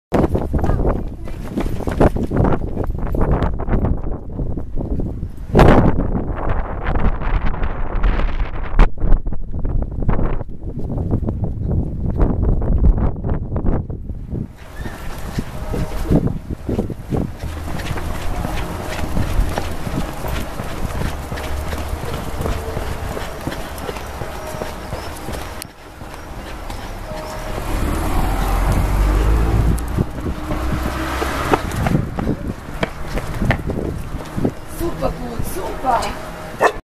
Ashanti Mantrailing Training 15.5.13
Ashanti wurde angereizt u.sieht ZP weglaufen, dann wurden ihr die Augen zugehalten, ZP dreht um und läuft auf der gegenüberliegenden Straßenseite wieder an Ashanti vorbei...
Temperatur 22°C, starker, böiger, sich drehender Wind